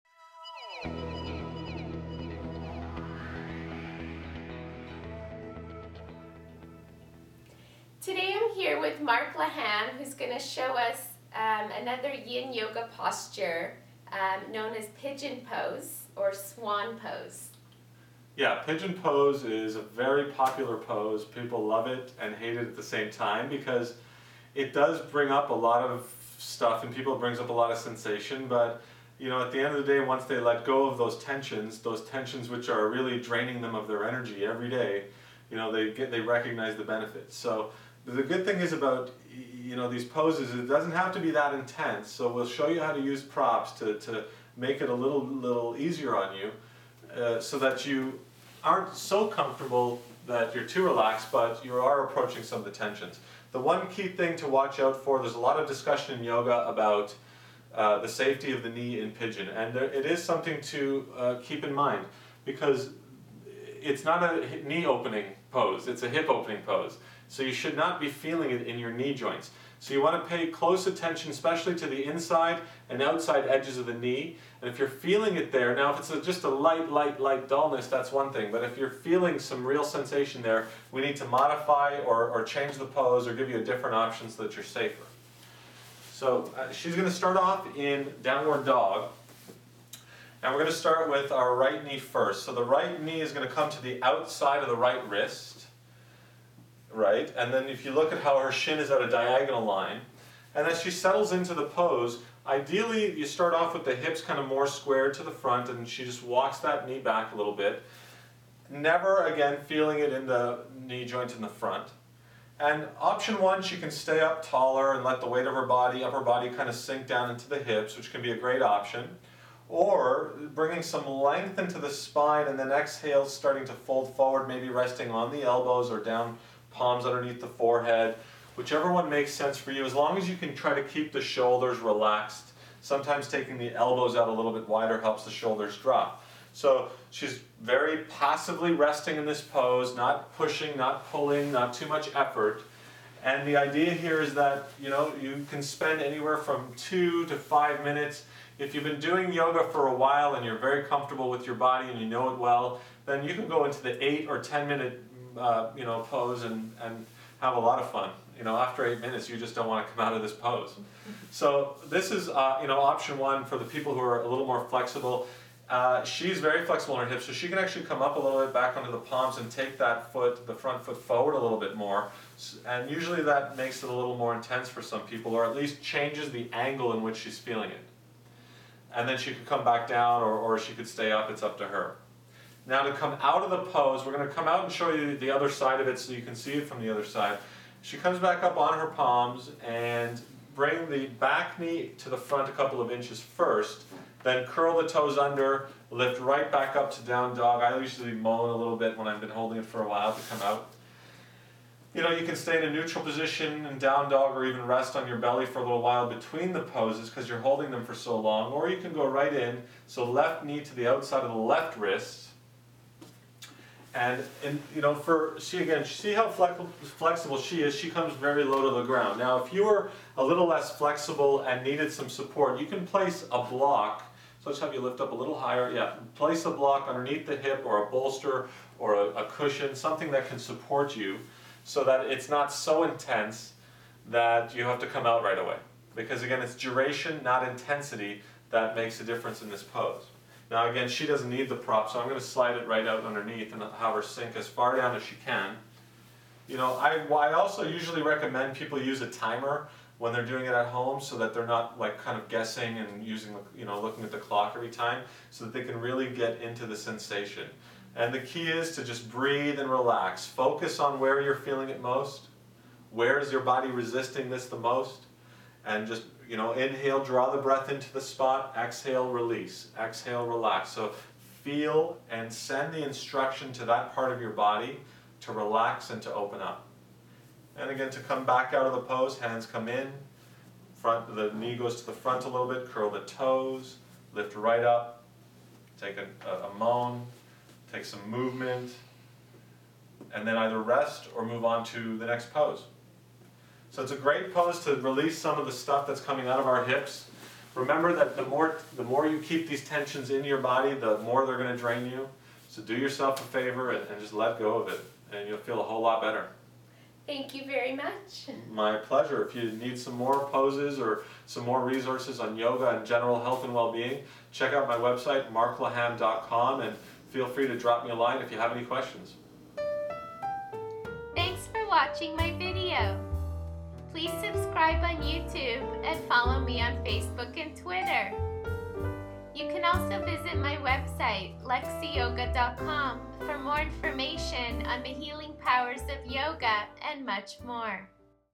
Yin Yoga Pigeon Pose
Yin Yoga involves holding onto the postures for a longer time. This enables you to reach into the connective tissues deep within your body. Even though yin yoga has a more restorative aspect, with time, you can gain more flexibility, strength and endurance.

In this video Mark Laham, (Yin Yoga instructor) is showing us some variations of Pigeon Pose, also known as Swan Pose.
Be sure to stay in this pose for at least 2-5 minutes for best results.